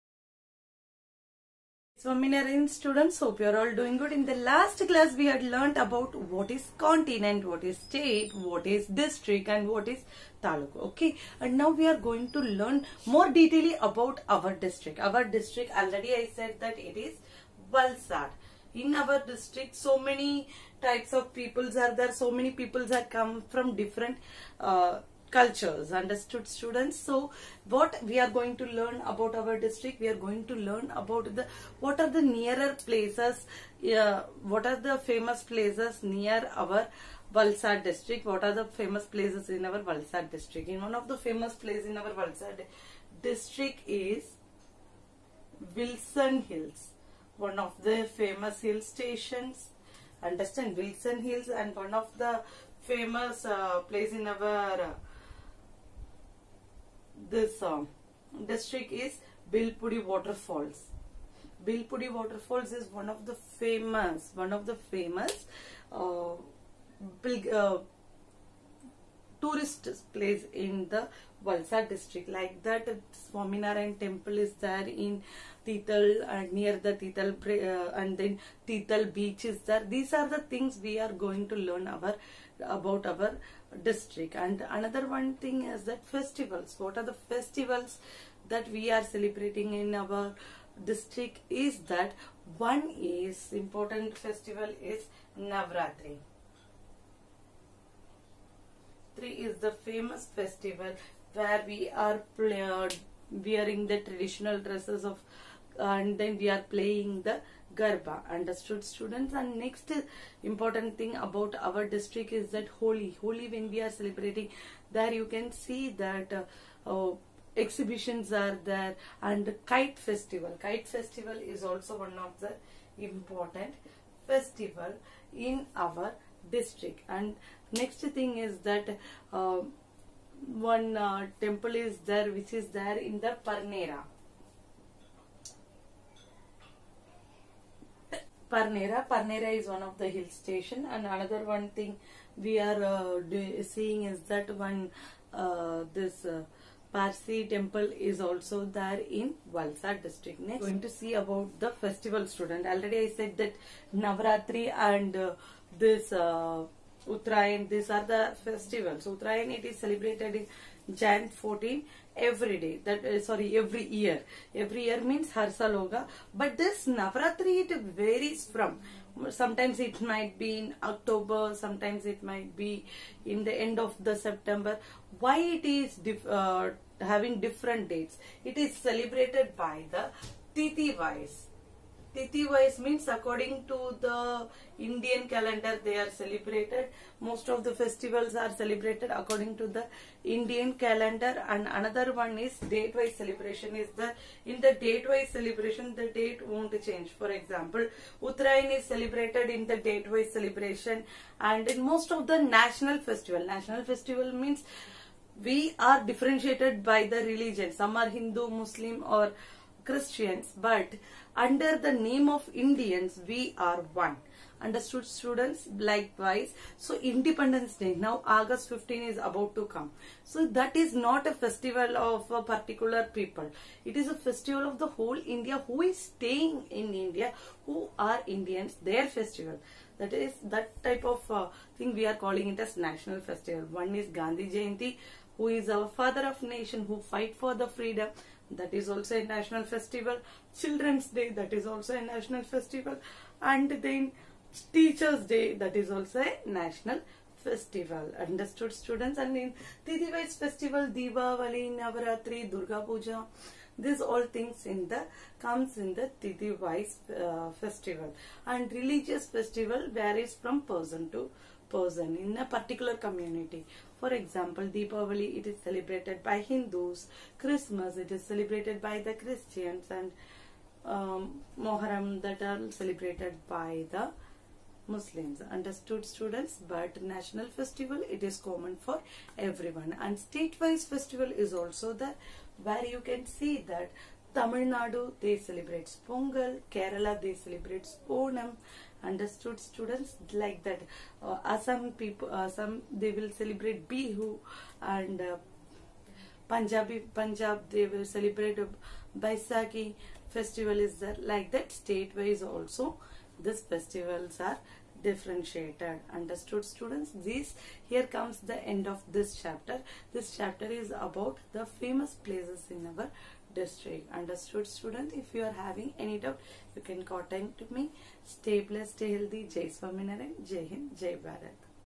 STD 5 EVS chapter 4 Our district part 2
explanation about the famous places in Valsad and national festival